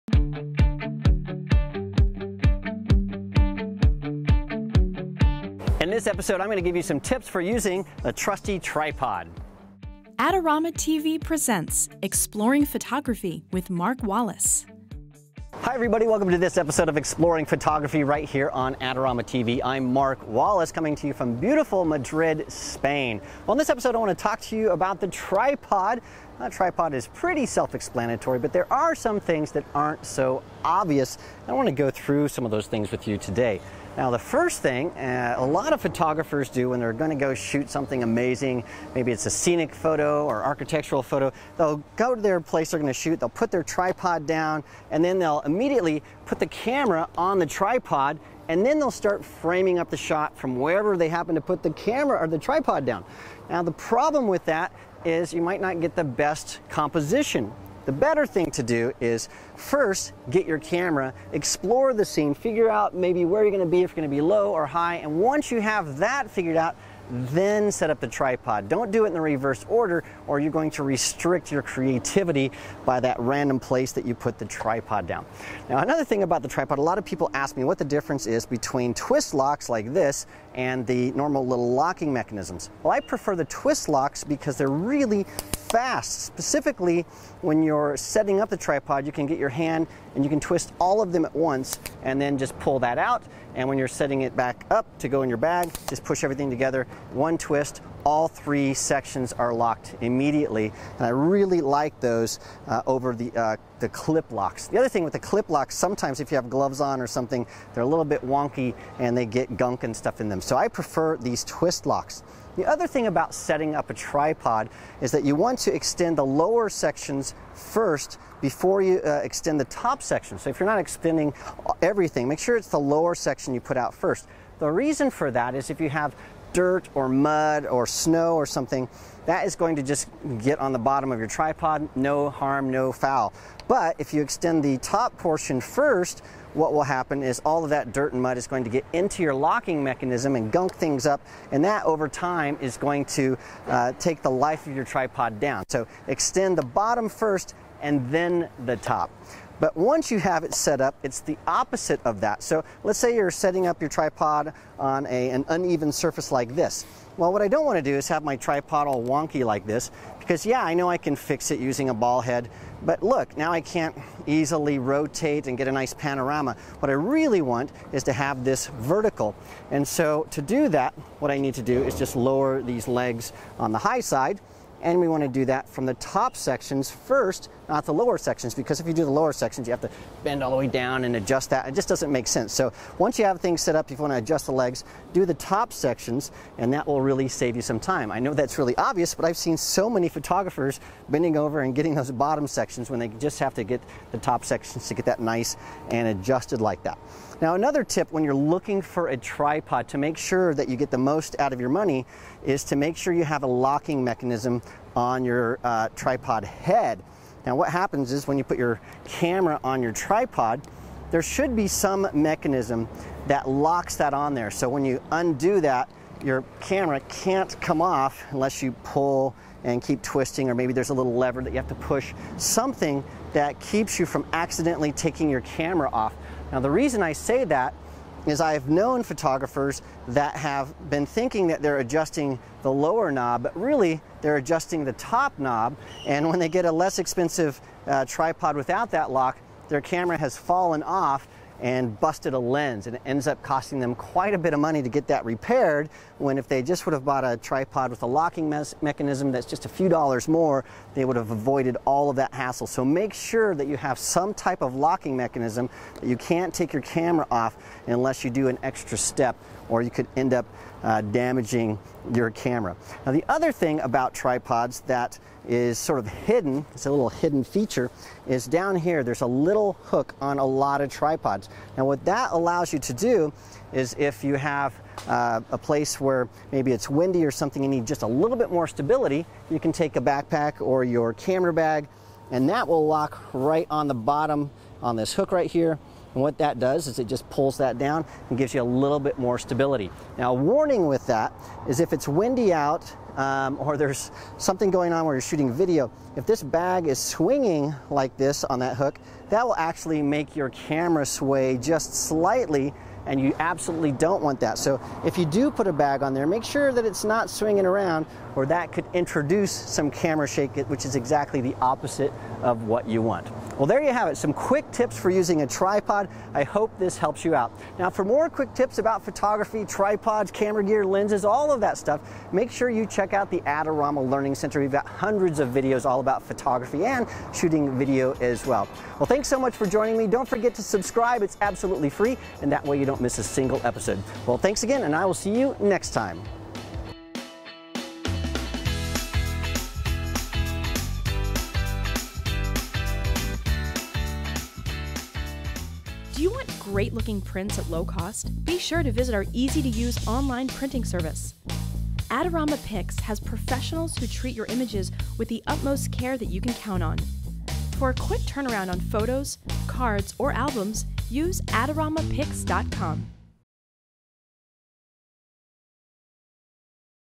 Tripod Tips: Exploring Photography with Mark Wallace
Using a tripod is pretty simple but there are some things that aren't so obvious. In this episode Mark Wallace gives viewers some basic tips for using a tripod.

Related Products at Adorama:

Benro Travel Angel 9X Carbon Fiber Series 2 Tripod Kit

Photos by Mark Wallace